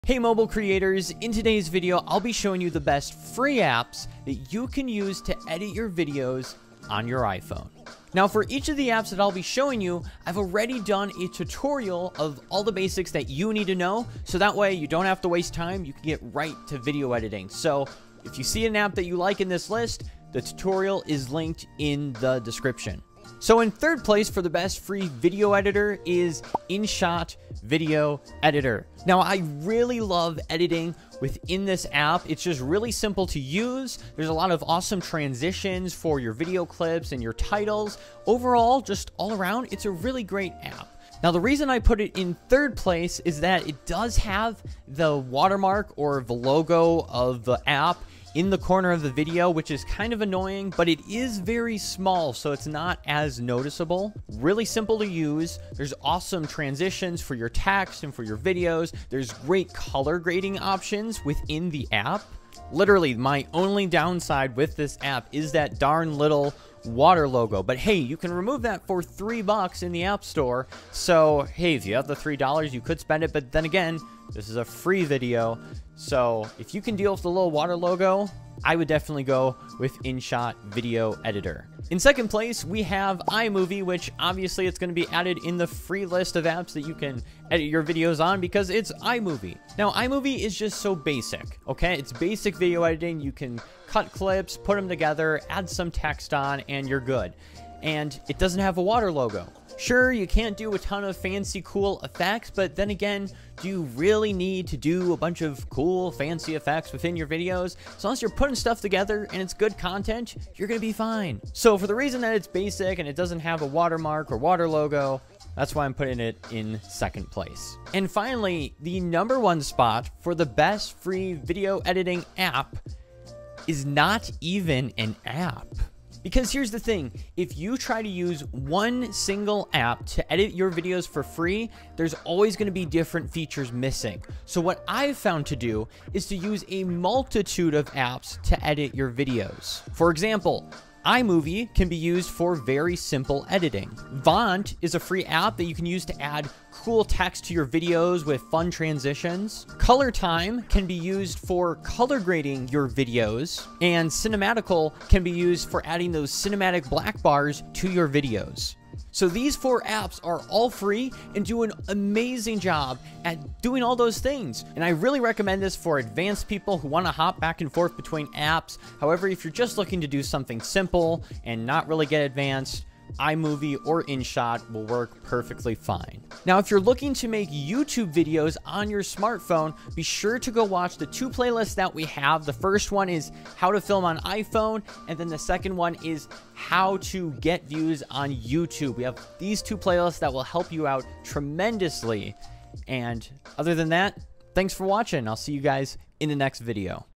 Best FREE Video Editing Apps on iPhone (2020)
Need a good free video editor for your iPhone or iPad? Here are 3 apps perfect for people on a budget and need good video edits.